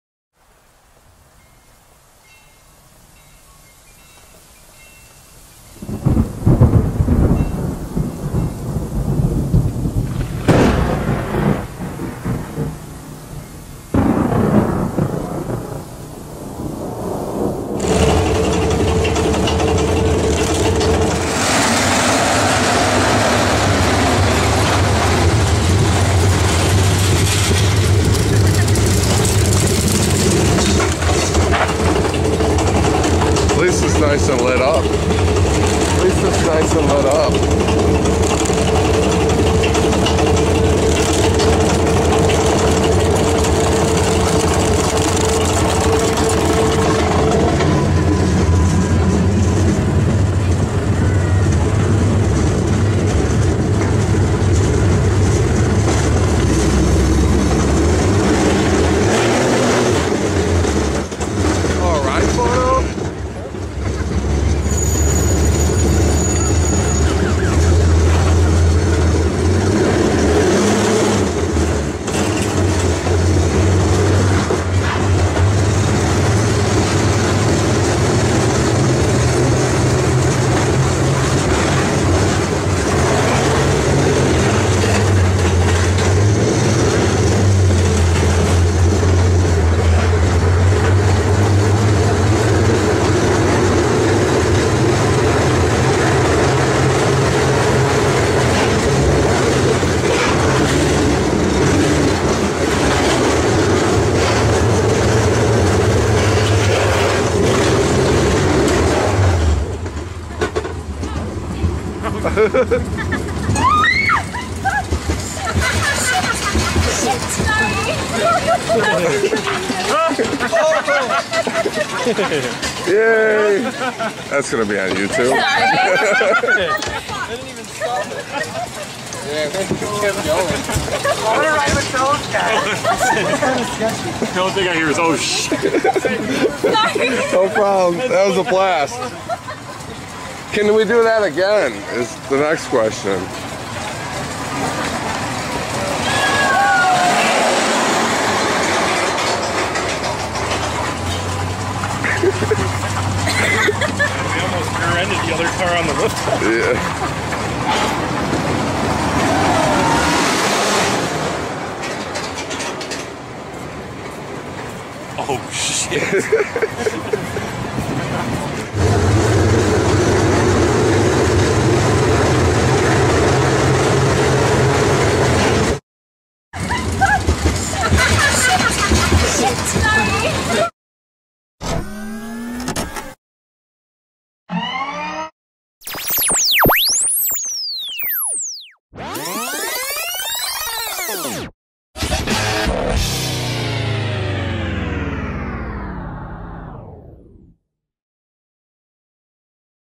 Sex, Drugs, and The Big OHHHH!
Watch as Rocco and the gang get the ride of their lives when the magnetic brakes fail on the Big Ohhh!. All is good and it made for one fantastic ride!!